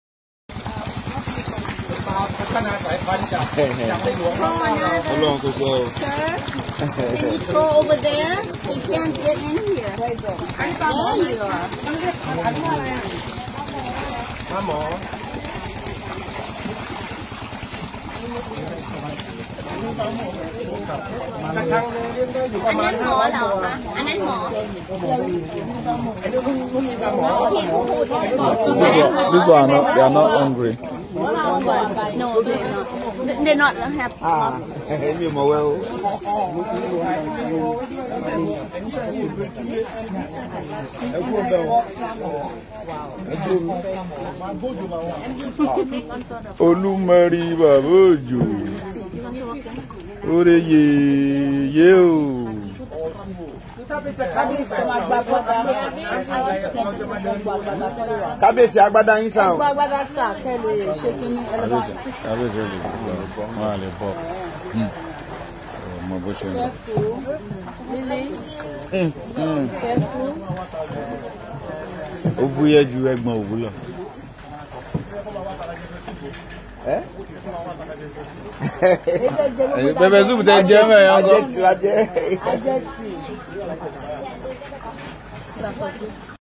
Day 2 of Iba Gani Adams, travel to see "Talad Thai", the largest agricultural market in Thailand.
Day 2 of Iba Gani Adams, The 15th Aare Ona Kakanfo of Yoruba land ' visit to Thailand falls on January 17, 2023. In the morning, travel to see "Talad Thai", the largest agricultural market in Thailand.